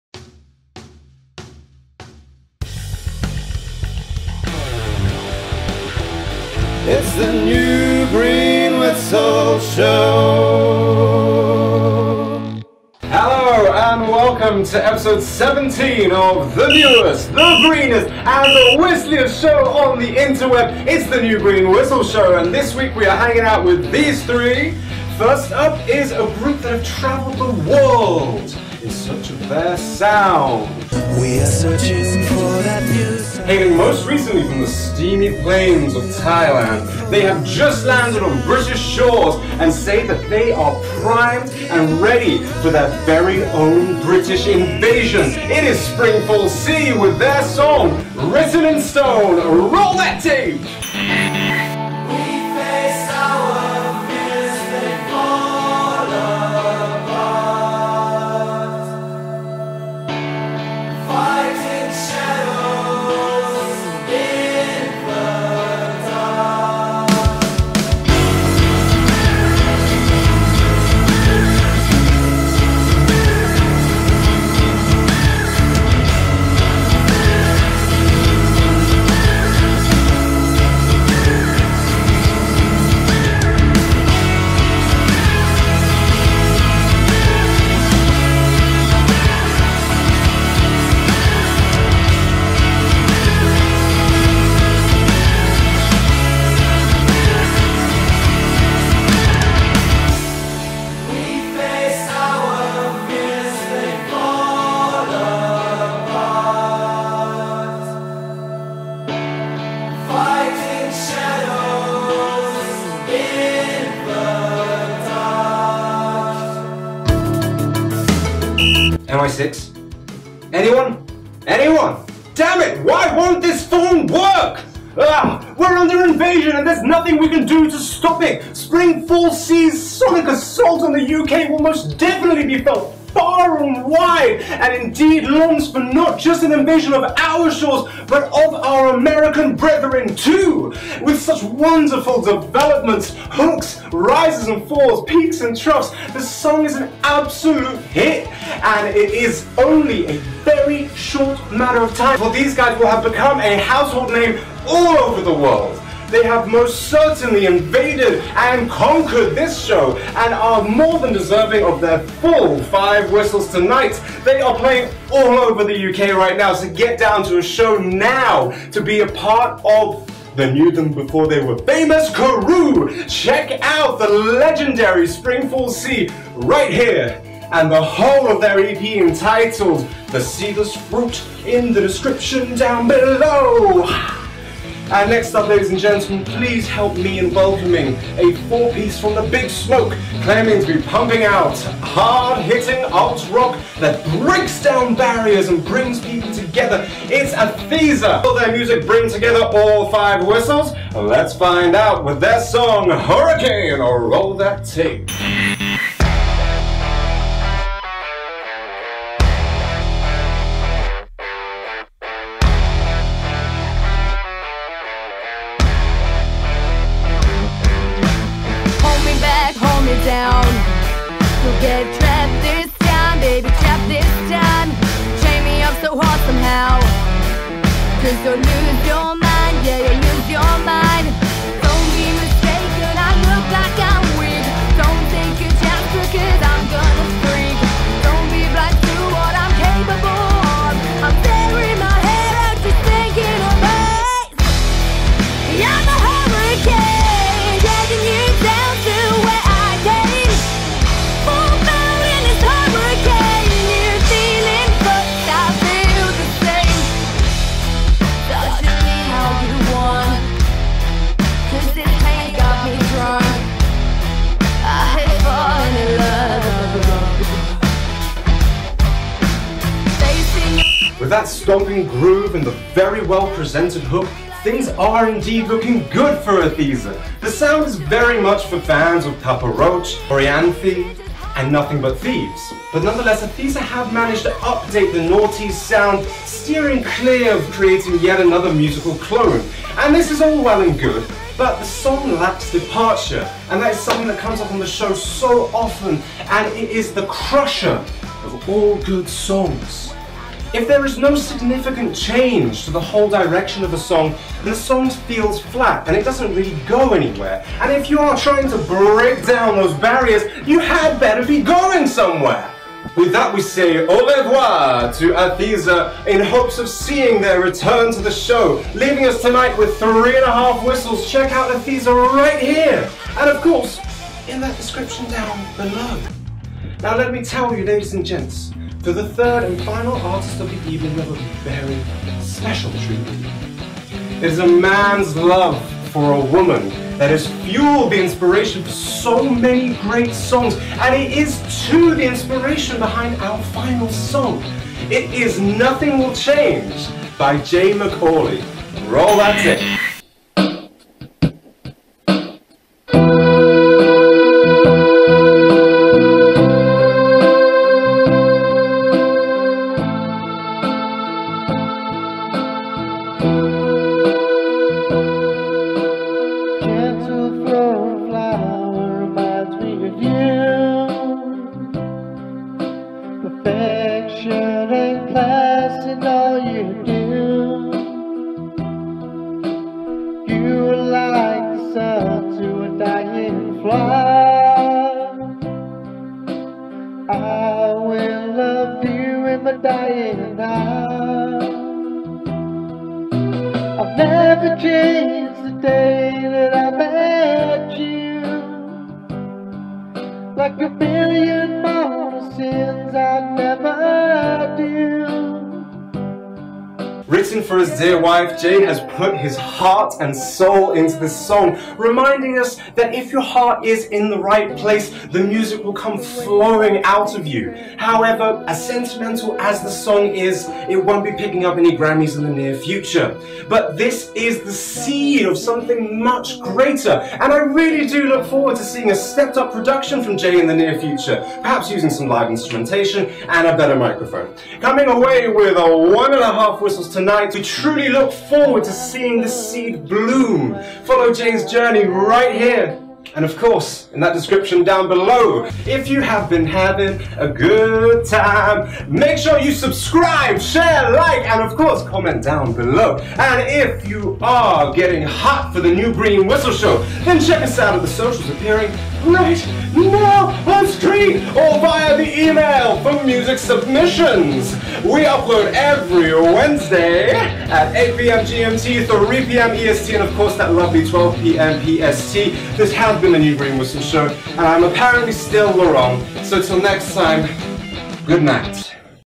The New Green Whistle Show Ep 17 Spring Fall Sea, Aetheza, Jay McAuley
All of this show's bands are listed below so go ahead check em out and don't forget to comment your submissions to be in with a chance of having YOUR music on the show!